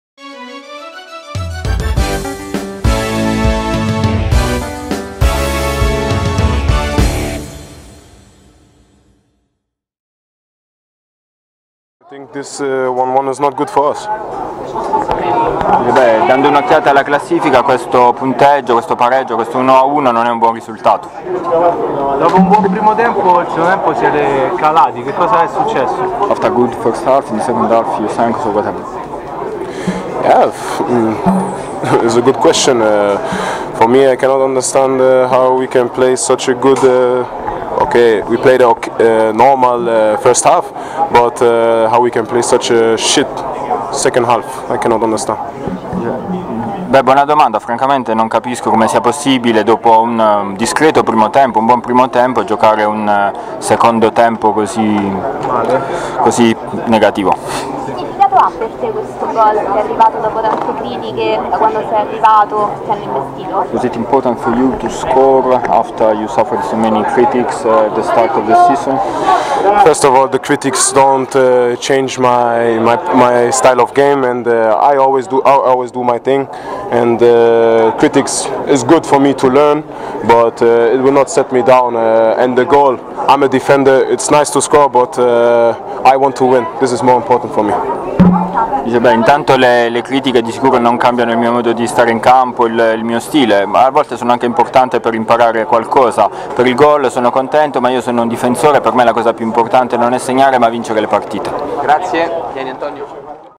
mixed Roma Milan SADIQ 09012016
Intervista al difensore della Roma - Umar Sadiq.
"Contento di aver segnato, sono un difensore, l'importante è vincere"
(intervista a cura di  Claudia Demenica)